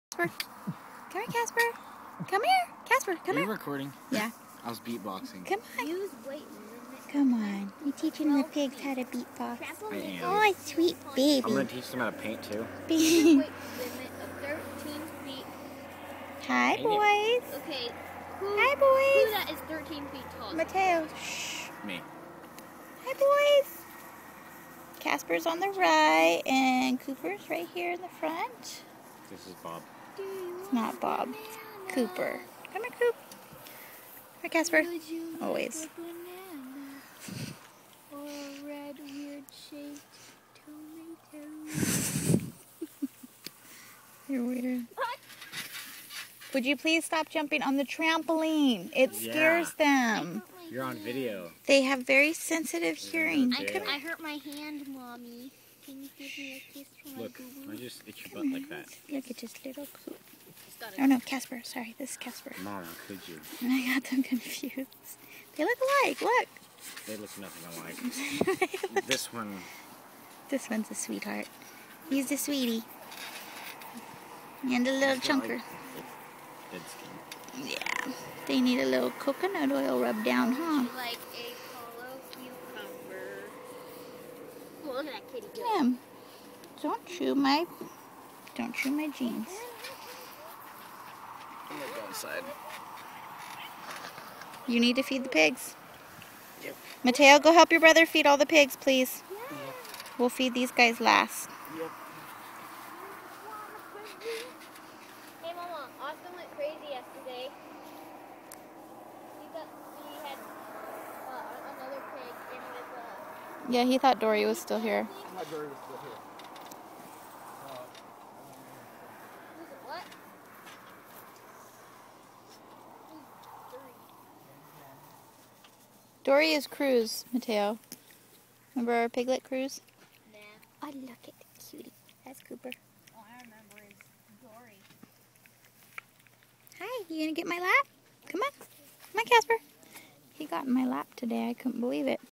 Casper and Cooper
Mini pig brothers, Casper and Cooper have some playtime with the family. They loved eating bananas and getting chin and belly scratches. They are both super sweet little boys at 8 weeks old. Casper is available for adoption.